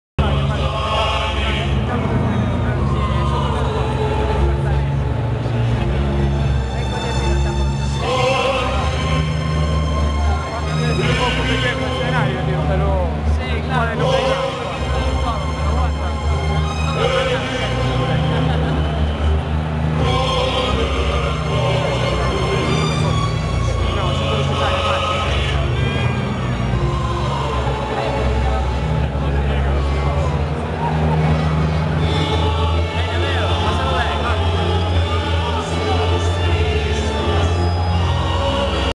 MACHINE HEAD & SLIPKNOT: LIVE IN SANTIAGO COMPOSTELA
Minutos antes del concierto del grupo de metal "Machine Head" en en el pabellón multiusos fontes  do Sar en Santiago de Compostela (Galicia).

07/07/09